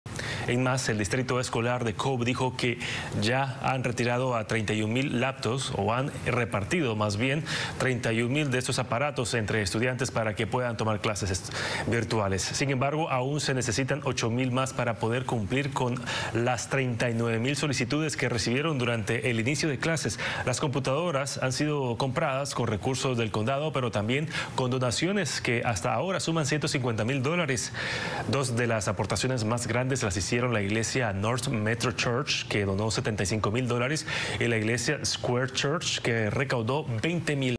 Cobb reparte 31,000 laptops; aún se necesitan 8,000 más para ayudar a estudiantes
El Distrito Escolar de Cobb dijo que ya se han repartido 31,000 computadoras portátiles entre estudiantes para que puedan tomar clases virtuales.